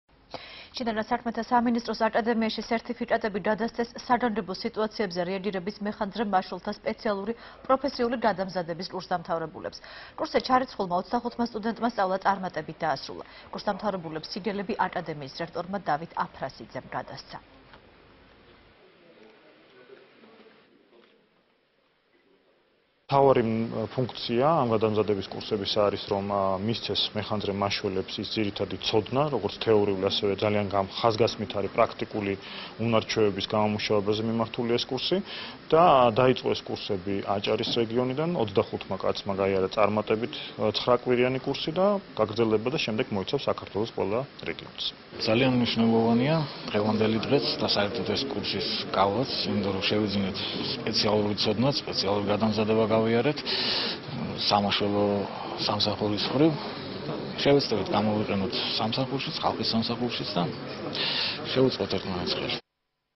ახალი 3 | პროფ.მომზადების კურსი | 07.07.12
შინაგან საქმეთა სამინისტროს აკადემიაში სერთიფიკატები გადასცეს საგანგებო სიტუაციებზე რეაგირების მეხანძრე-მაშველთა სპეციალური პროფესიული გადამზადების კურსდამთავრებულებს. კურსზე ჩარიცხულმა 25-მა სტუდენტმა სწავლა წარმატებით დაასრულა. კურსდამთავრებულებს სიგელები აკადემიის რექტორმა, დავით აფრასიძემ გადასცა.